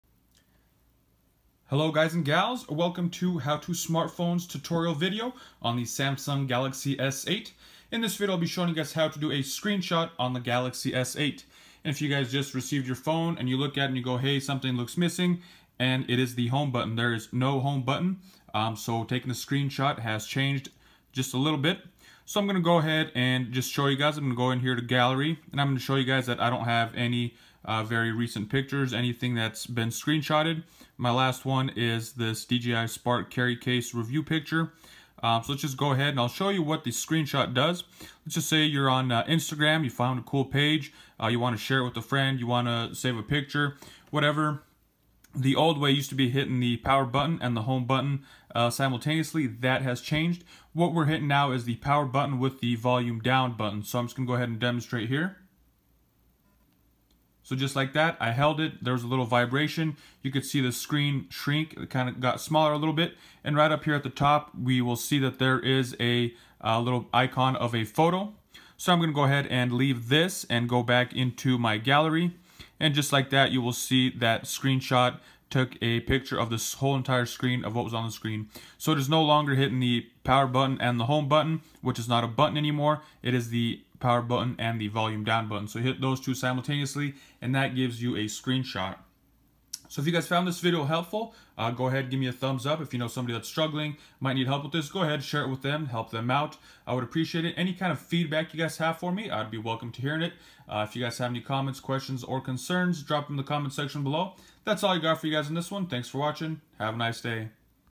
How to Screenshot on Samsung Galaxy S8
How to screenshot on a Samsung Galaxy S8. This tutorial video shows how to do a screenshot on the Galaxy S8. Without the home button the way to screenshot has changed.